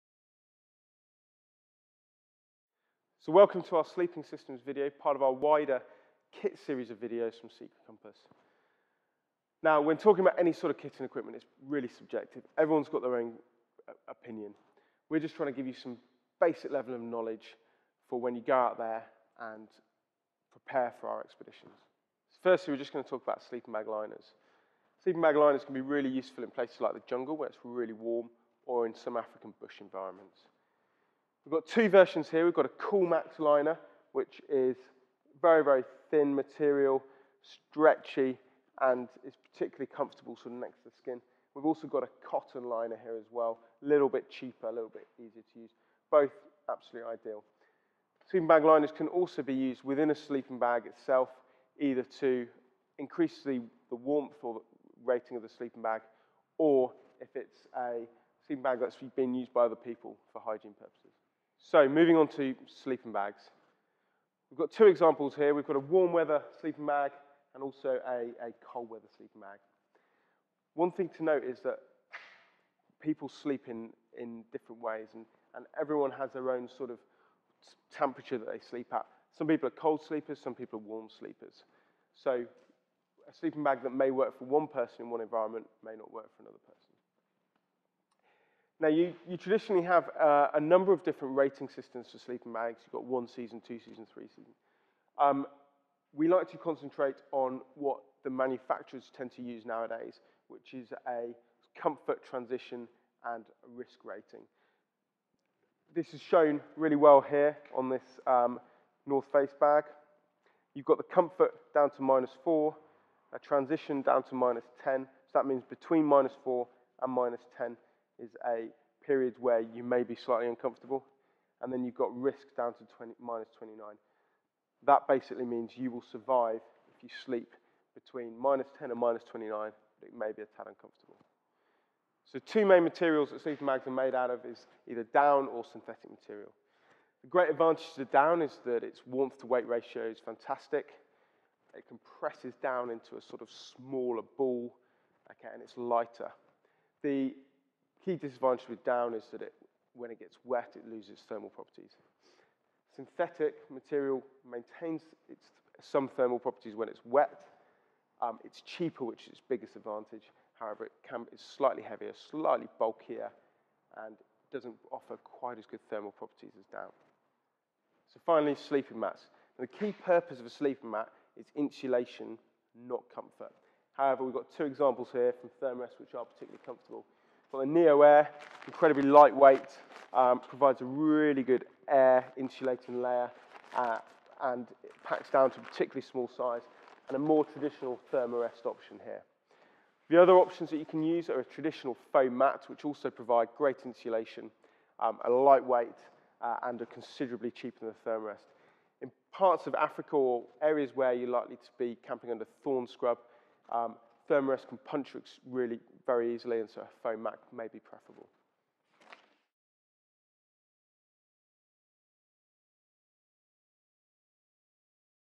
Secret Compass Expedition Kit Advice: Sleeping bags & ground mats
Expedition expert Tom Bodkin talks you through the different types of sleeping bag, sleeping bag liner, grading systems and inflatable/ foam ground mats in this Expedition Kit Advice video from adventure company, Secret Compass.